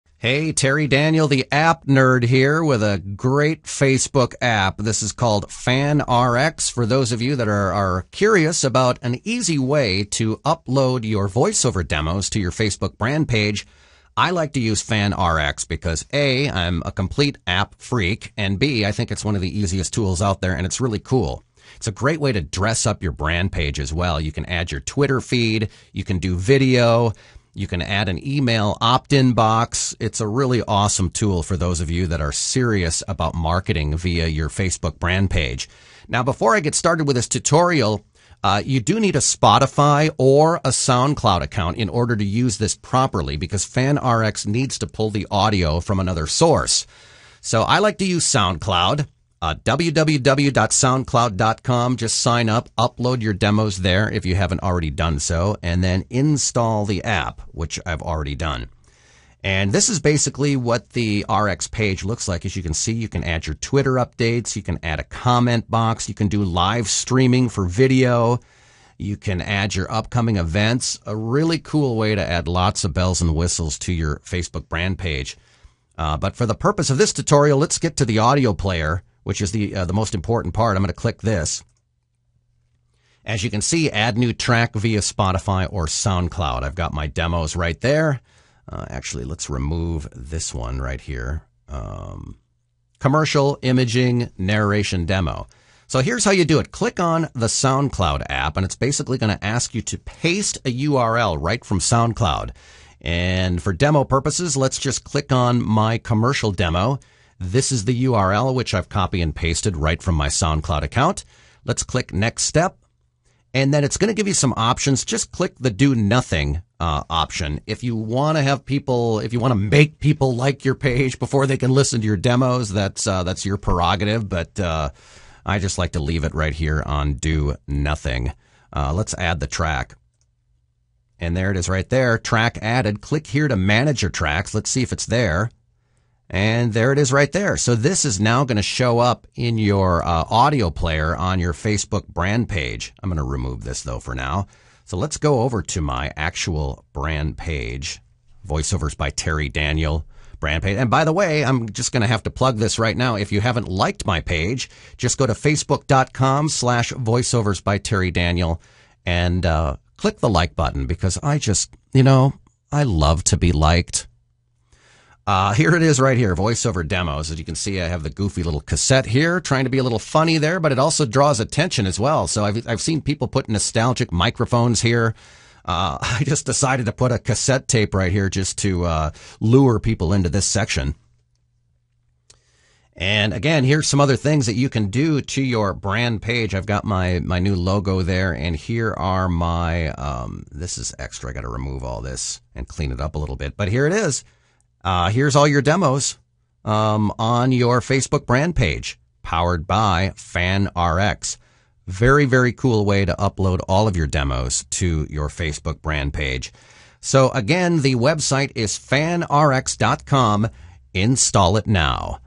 How To Use FanRx On Your Facebook Brand Page
Here's a few tips on how to use FanRx to dress up your facebook brand page including adding your demos!

For more information on voiceover training, please visit my